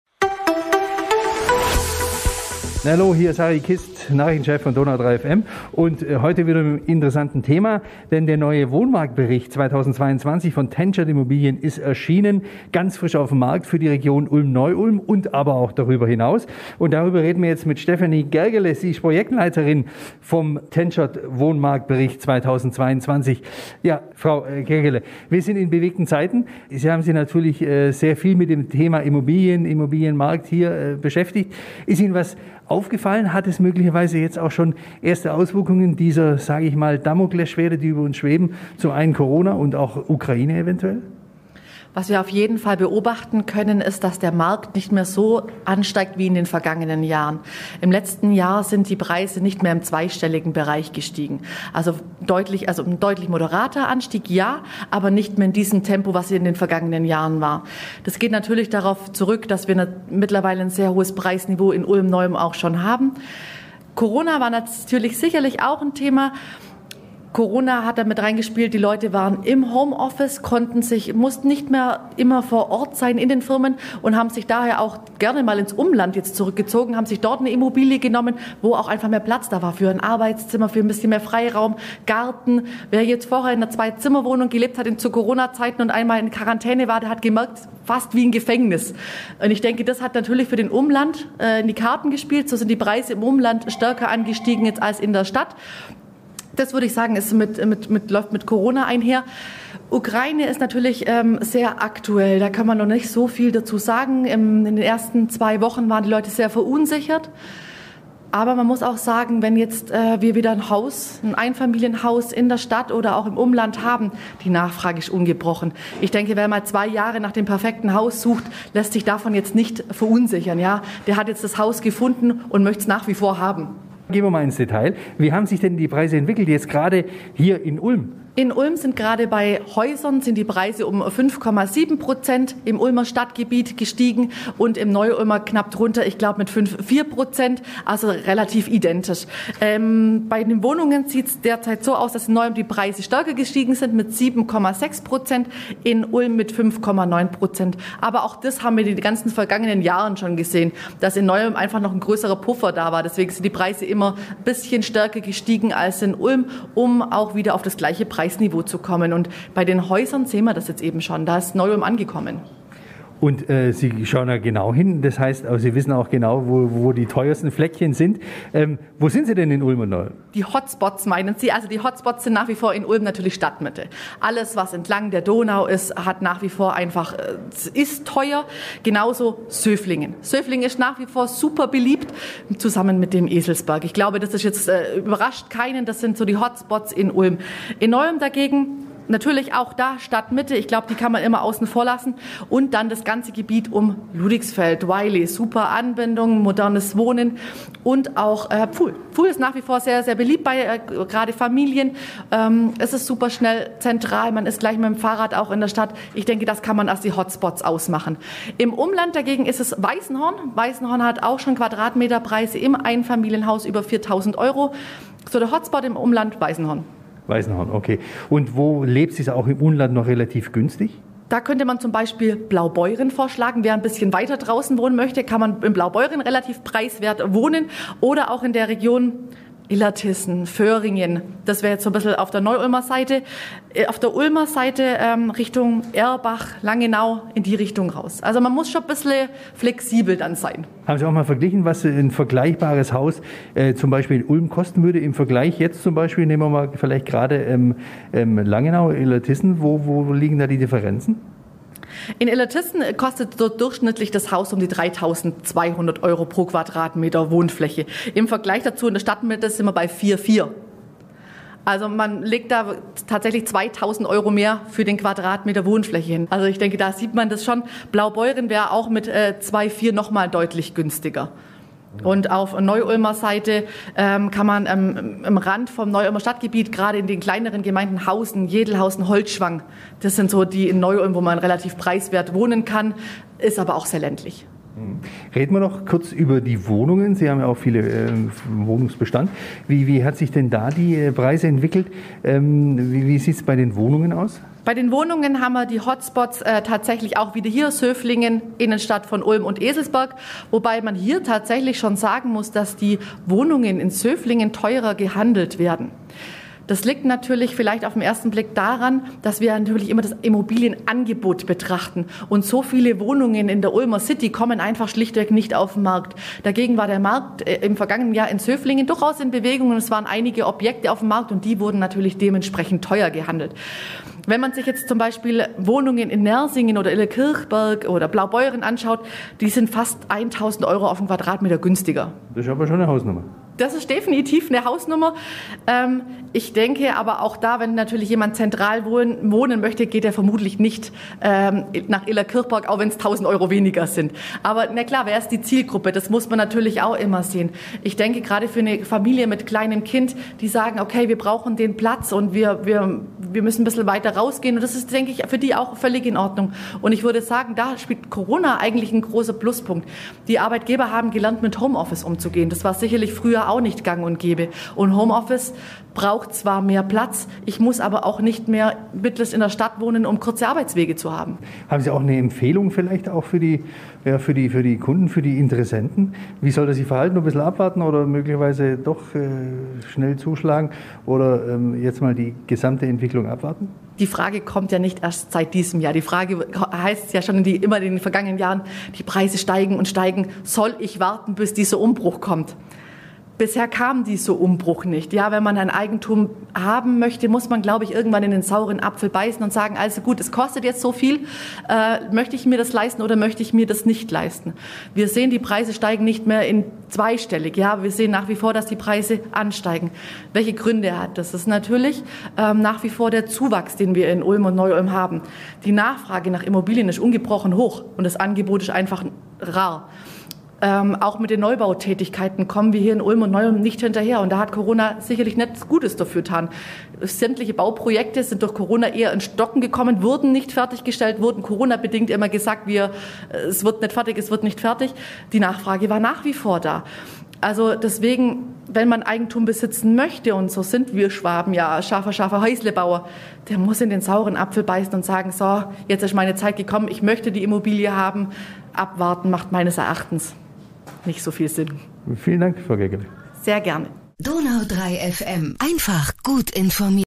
Tentschert Immobilien Ulm: Neuer Wohnmarktbericht ist da! - Schwabenreporter
Die Nachfrage nach Häusern und Wohnungen im Raum Ulm ist nach wie vor ungebrochen hoch. Das geht aus dem neuesten Wohnmarktbericht von Tentschert Immobilien für Ulm, Neu-Ulm und die Region hervor.

Zum 9. Mal in Folge veröffentlicht Tentschert Immobilien seinen Wohnmarktbericht. In Kooperation mit dem renommierten iib-Institut (Institut Innovatives Bauen) sind in der Broschüre (Auflage 8.500 Exemplare) wieder die aktuellsten Zahlen und Fakten sowie die Preisentwicklungen für den regionalen Immobilienmarkt zum Stichtag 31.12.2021 übersichtlich, nach Stadtteilen und Ortschaften aufgeteilt, dargestellt. Der aktuelle Wohnmarktbericht kann bei Tentschert Immobilien in Ulm angefordert werden.

Stefanie Gergele, Projektleiterin bei Tentschert Immobilien, hat sich mit DONAU 3 FM Nachrichtenchef Harry Kist über die aktuellen Entwicklungen, über teure Pflaster und günstiges Wohnen unterhalten.